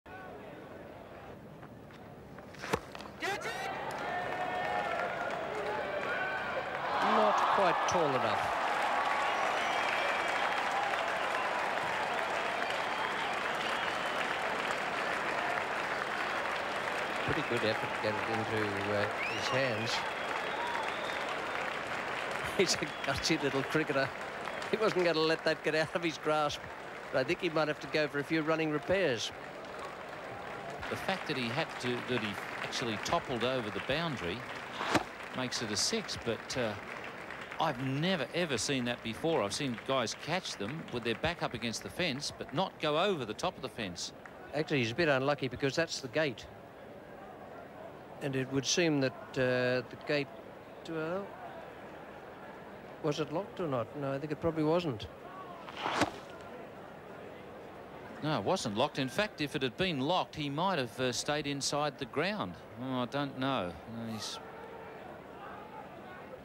Saeed Anwar takes on fence in all-time painful catch attempt | From the Vault
How about the commitment! Unfortunately, Saeed Anwar's classic catch attempt wasn't enough to prevent six runs and a few bruises at the Adelaide Oval in 1989/90.